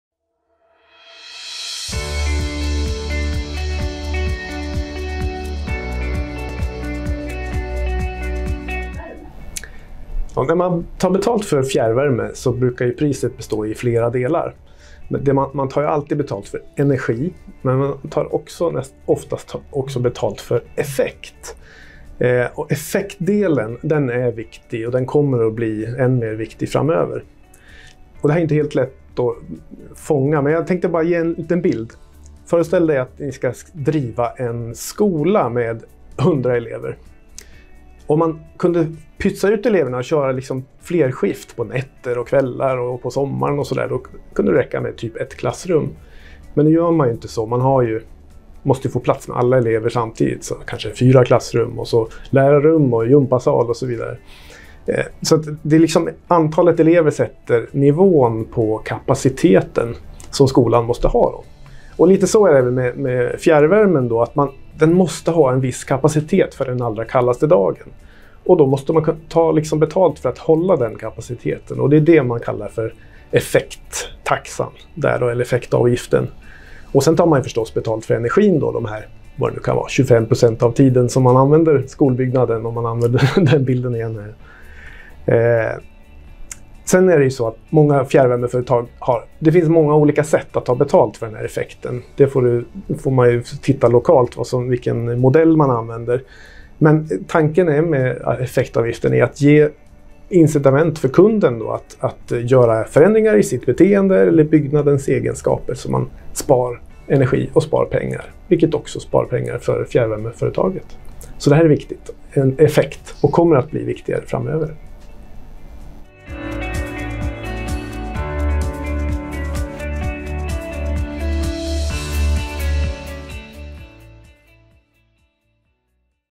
Vad innebär effektavgiften i fjärrvärmen?
Varför betalar jag för både effekt och energi på fjärrvärmefakturan? Daniel Lundqvist på Energiföretagen Sverige förklarar.